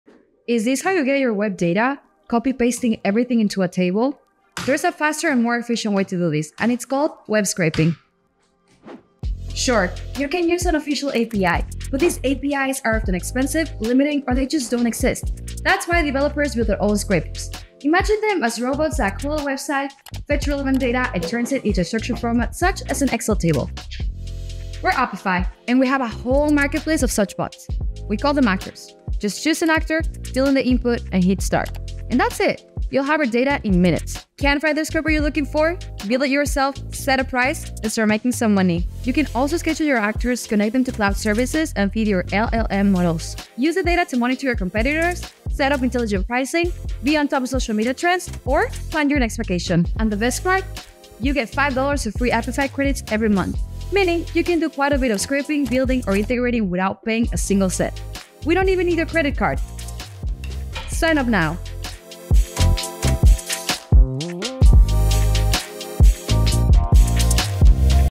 Try Apify for Free - 10,000+ Pre-built Web Scraping and Automation Tools
Apify is the platform where developers build, deploy, and publish web scraping, data extraction, AI agents, and web automation tools. Choose from over 8,000+ pre-built APIs in Apify Store - our growing automation marketplace 🤖

Both dev and noob-friendly 🧑‍💻
Actors (as we like to call them) feature an auto-generated UI for setting their input, meaning you don’t have to be a dev to scrape the web.

Integrations. Everywhere. 🧩
Further automate your workflow by scheduling or integrating your Actors. Connect them with platforms such as n8n, Zapier, Make the Google Suite, GitHub or feed your LLMs through LangChain, LlamaIndex or MCP.

Can’t find an Actor? Build it yourself from one of our web scraping templates or import existing code from a Git repo in any programming language. Publish your project in Apify Store and earn passive income.

Never get blocked 🚫
Use our large pool of datacenter and residential proxies. Rely on smart IP address rotation with human-like browser fingerprints.